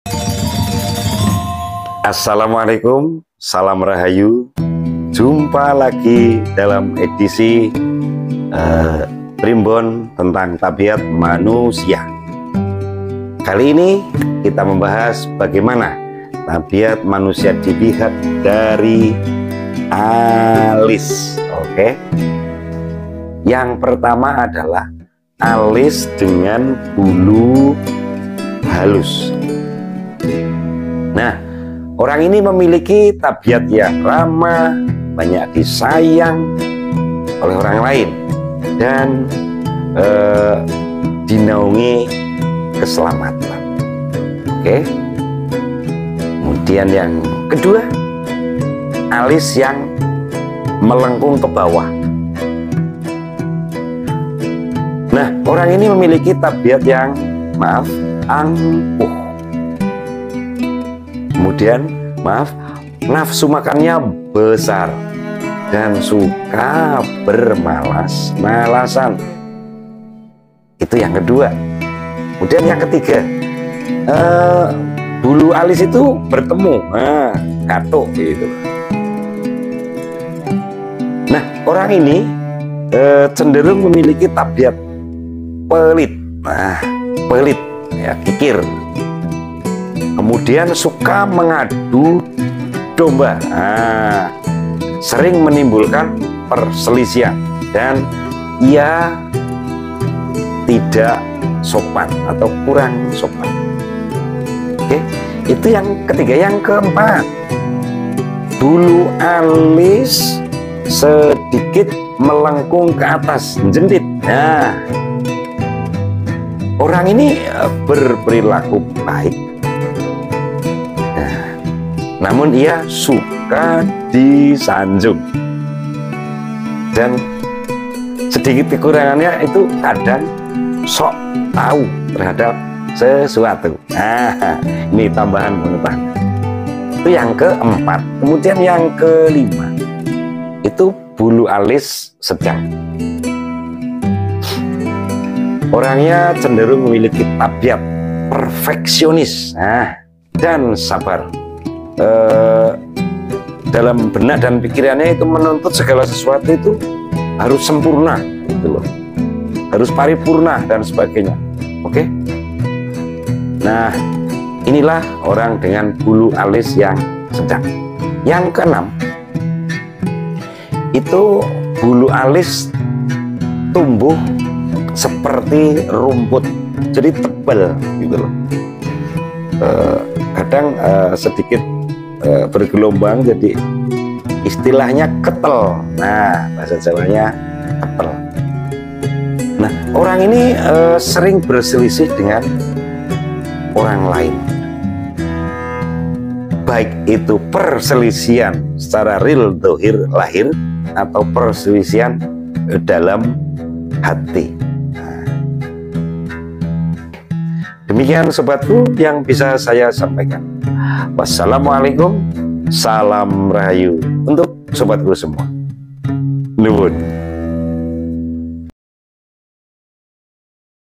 6 Arti Bentuk ALIS MATA Manusia - part 1
Primbon Jawa menyimpan rahasia watak, tabiat, karakter, dan nasib manusia yang dilihat dari tampilan alis mata orang yang menyandangnya. Ada alis halus, alis melengkung, alis rimbun, alis sedang, kemunculan tanda di dekat alis mata, dan sebagainya. Semua itu memiliki watak, tabiat, karakter, dan nasib berbeda bagi yang punya.

Selamat menyimak semoga mendapatkan manfaat!